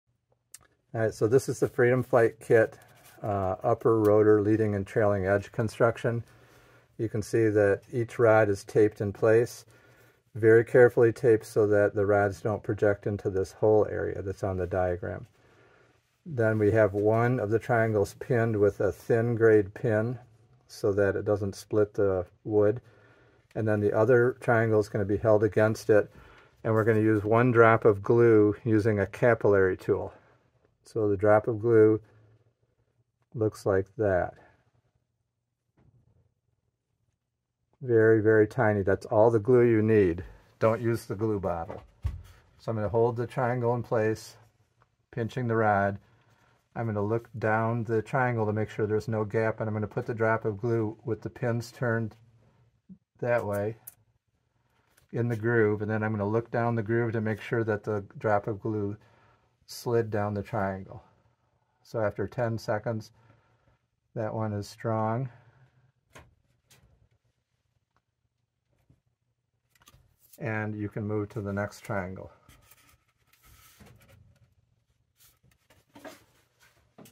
2024 Freedom Flight Helicopter Top Rotor Build Demo Step 1
Rotor Spar construction demo.